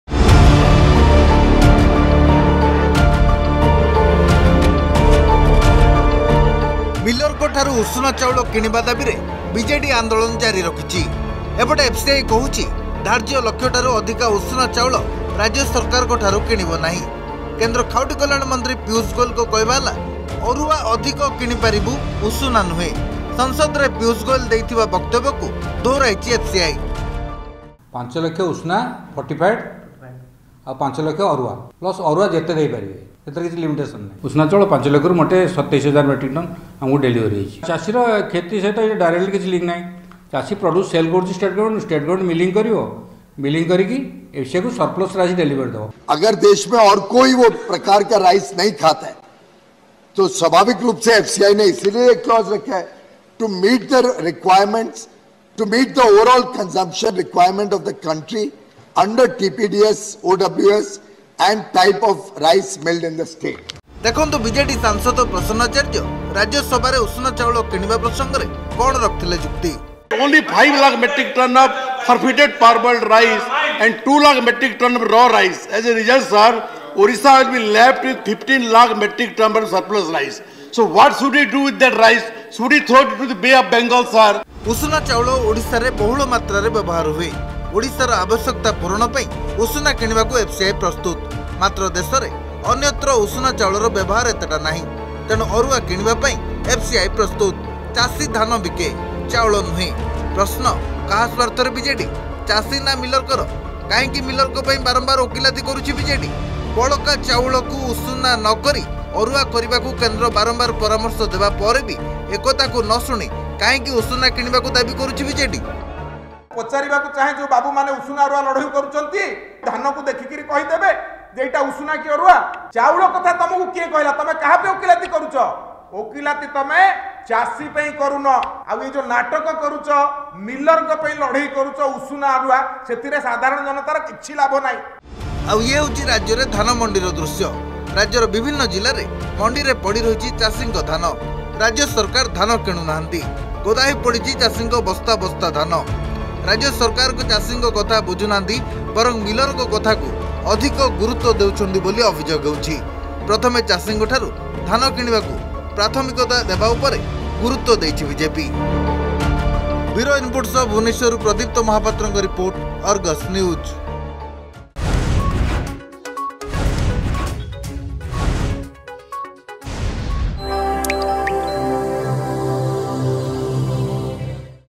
BJD is Resorting to Protest over Arua-Usna Rice for No Reason
The ruling BJD party and the opposition BJP are at loggerheads over the issue. The BJP has lashed out at the state government in response to allegations that the BJP did not buy the Usuna rice .